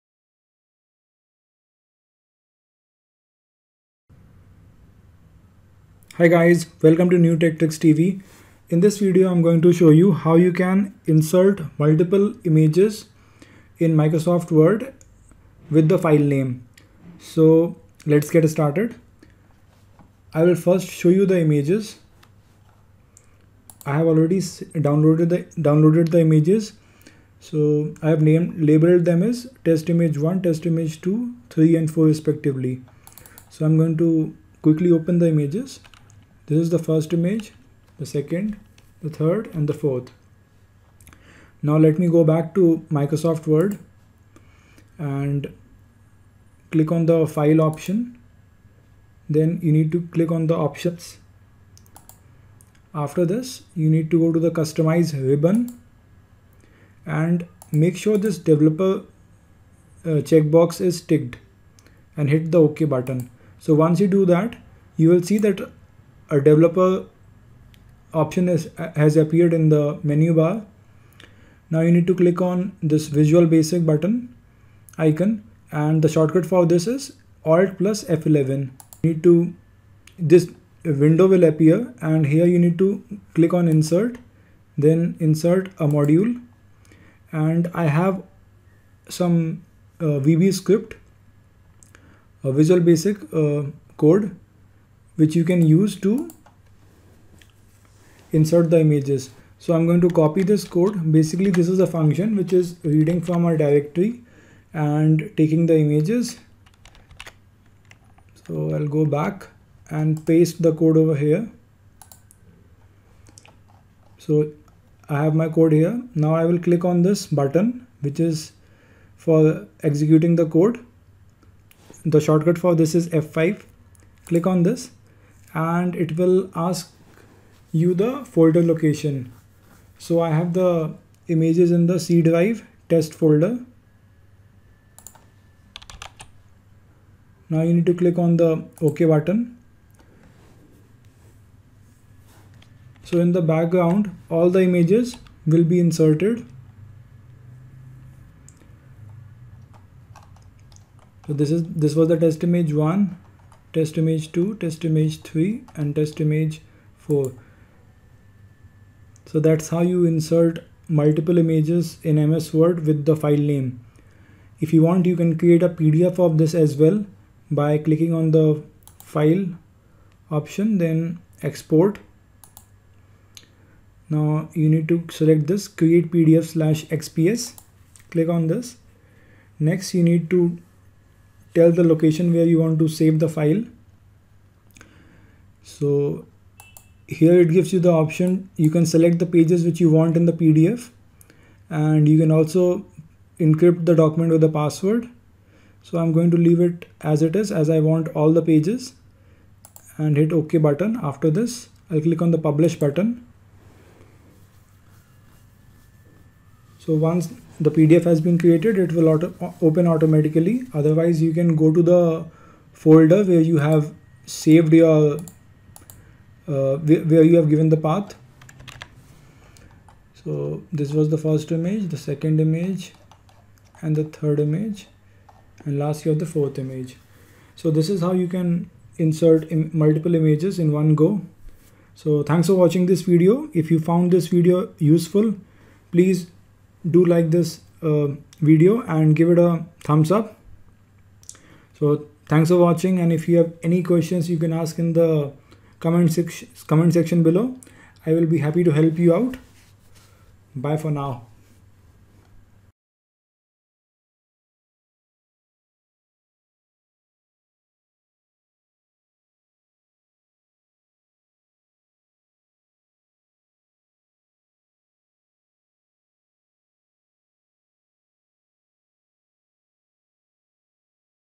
How to insert multiple images with filename in Microsoft Word (Windows) ?
How to insert multiple images with the filename in Microsoft Word using VBScript?
Software used in the video - Microsoft Word 2016